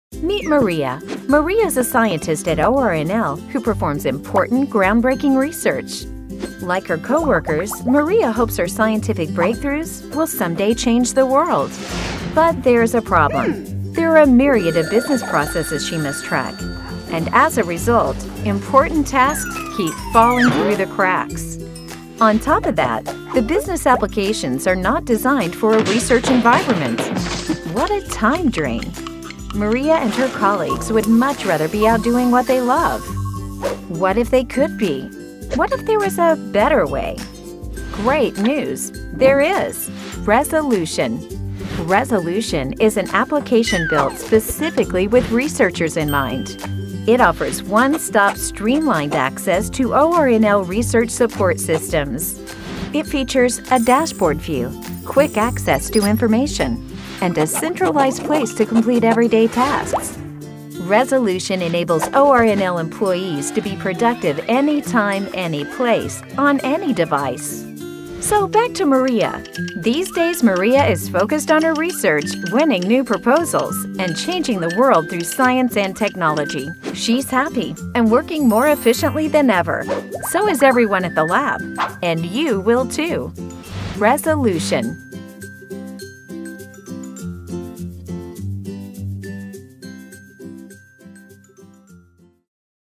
Meet Maria, a researcher who loves RESolution
RESolution is an ORNL-developed application designed to simplify researcher interaction with other ORNL computing systems and provide project management tools tailored for the research environment. See how RESolution helps researchers spend less time on administrative tasks and more time on research.